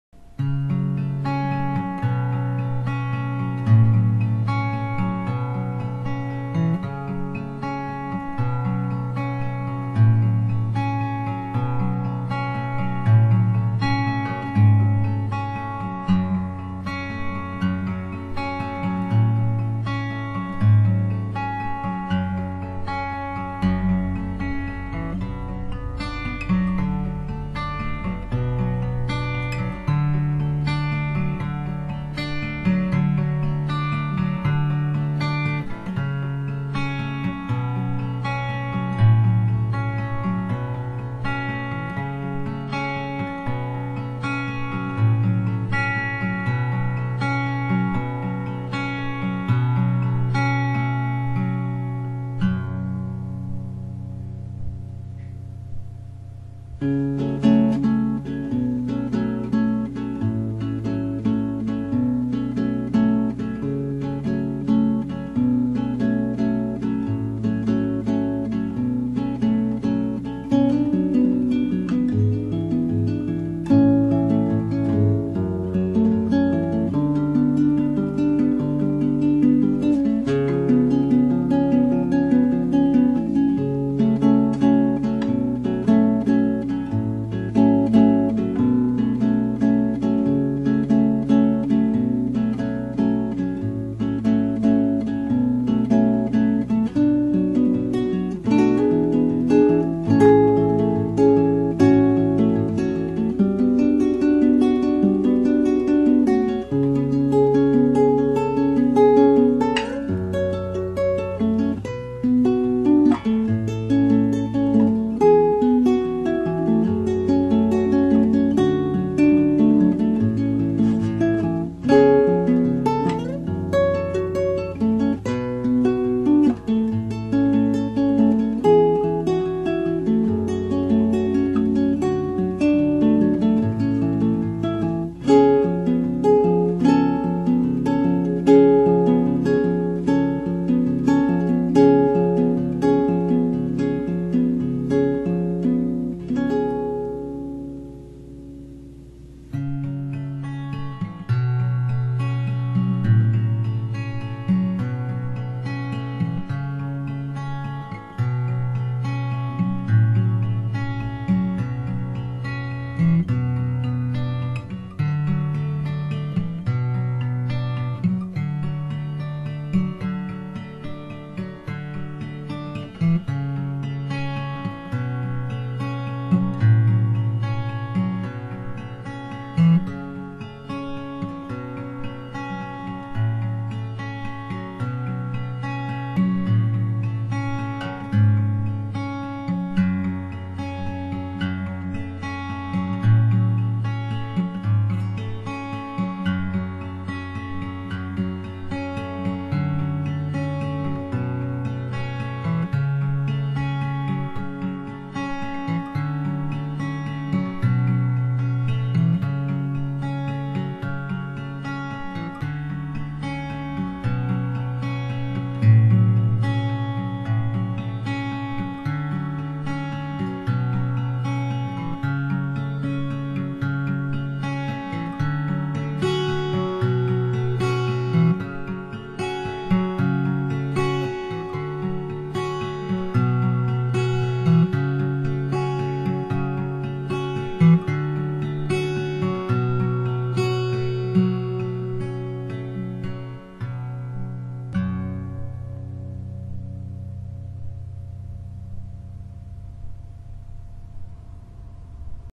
jagrick
Just 2 friends from different parts of the world on guitar with our own composition.